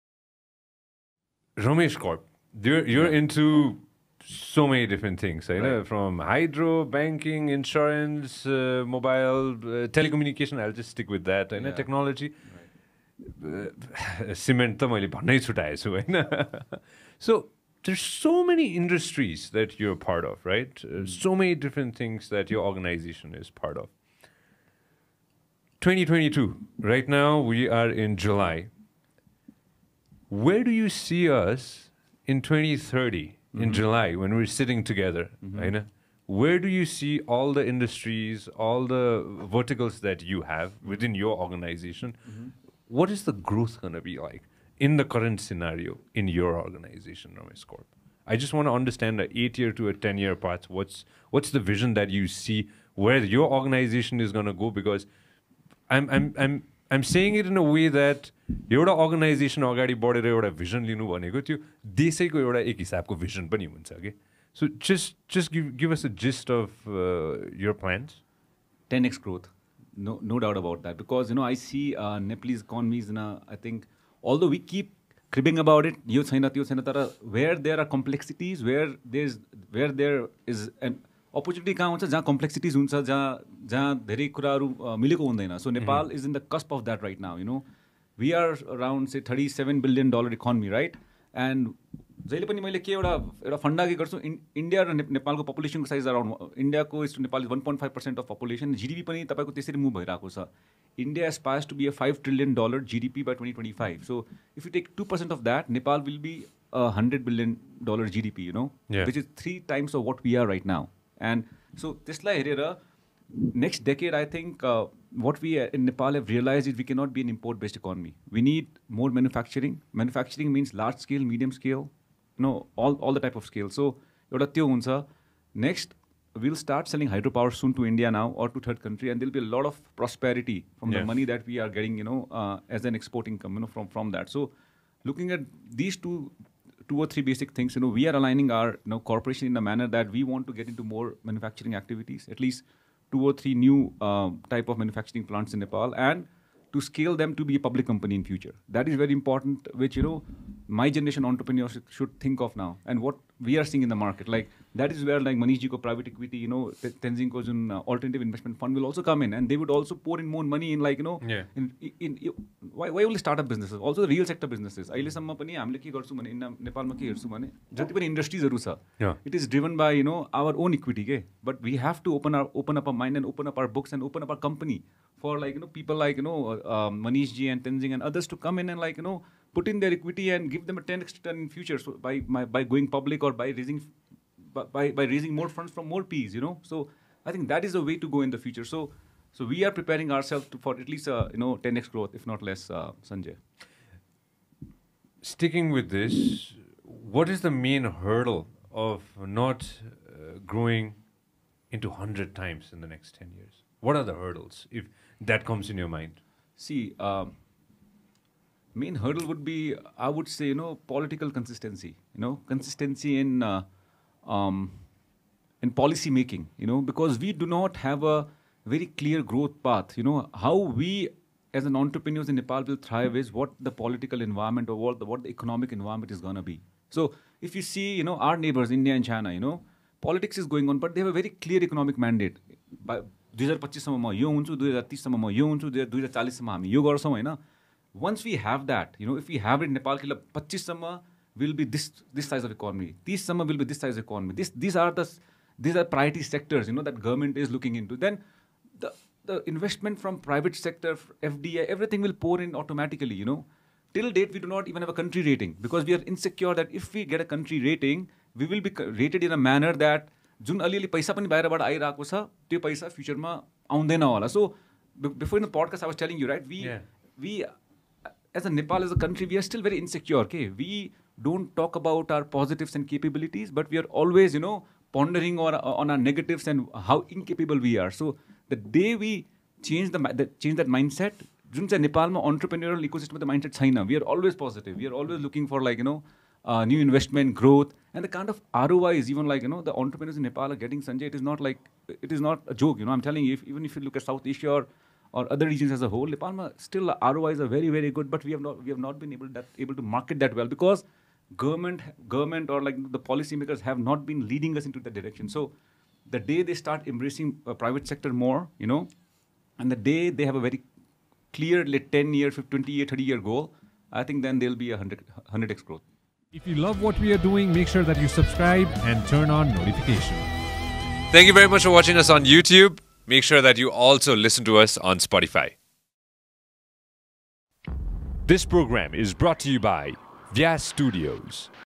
Benefits and Drawbacks Of Entrepreneurship in Nepal | Rohit Gupta
Dr. Manish Thapa is the founding managing partner of Global Equity Fund, BOD of Laxmi Bank, and the founding member of the Department of Conflict, Peace & Development Studies. Rohit Gupta is the vice-chairman of Ramesh Corp, co-founder of Tele Ventures, and National Council Member of CNI. Manoj Paudel is the founder and managing partner of Aadhyanta and had positions at Oxford Brookes University, Grant Thornton UK LLP, and more. In addition to being the CEO and creator of Team Ventures, Tenzin Sonam Gonsar also founded SDI Nepal and serves as an alternate director for the Nepal Warehousing Company. And Mooja Manandhar is the head of Finance & Corporate Advisory at Himalayan Capital Ltd, previously Senior Audit Associate of KB Chitracar & Co. In this episode of On Air, we talk about Merchant banks, Mahabir Pun, investment in Nepal, innovation, economic stature, Balen, Private Equity, and many more of their agendas.